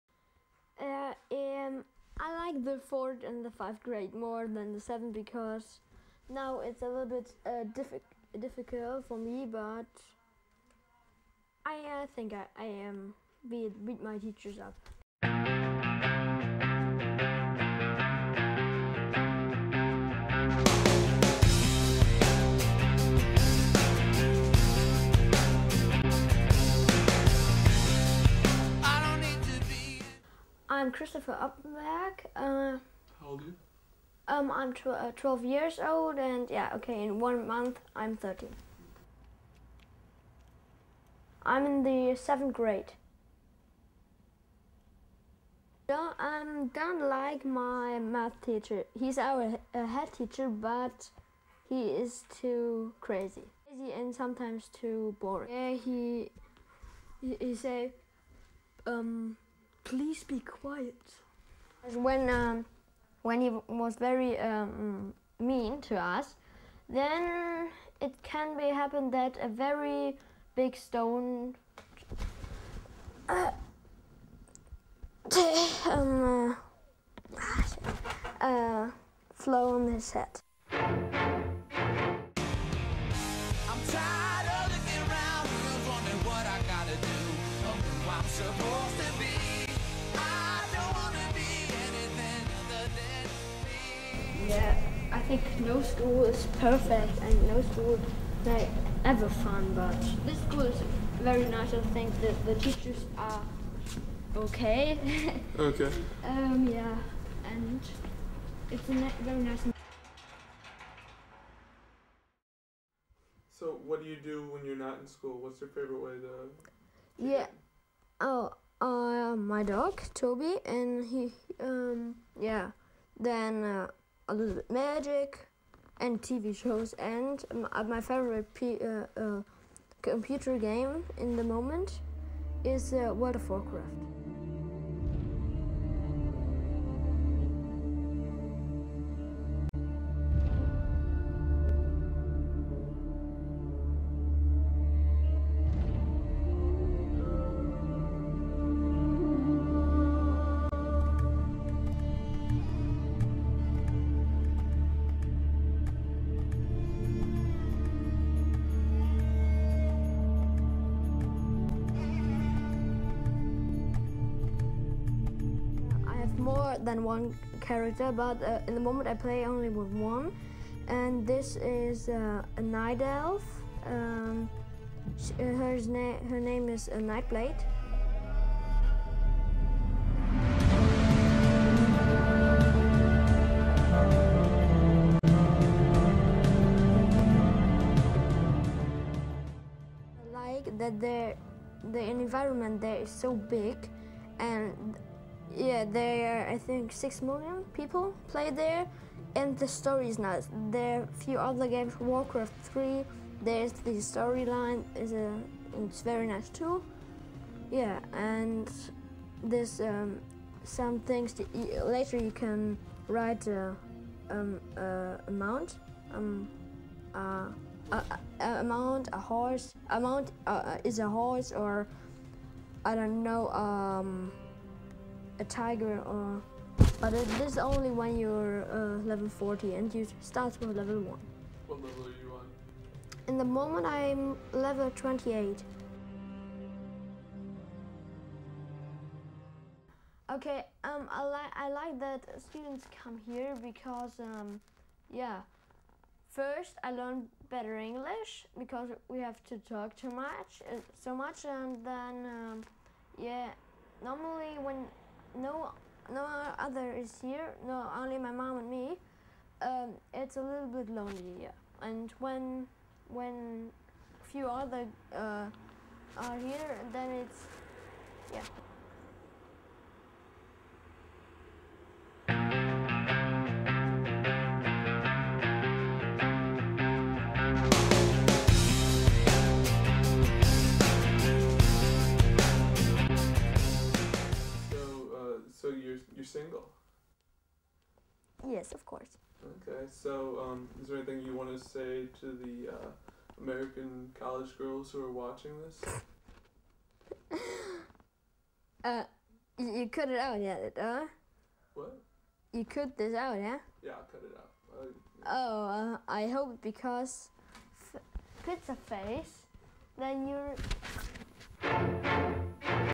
Ein Schlitzohr Named Kristoffer
my final product for my editing class... a portrait of my host mother's son Kristoffer... messed up by the compressor used to export it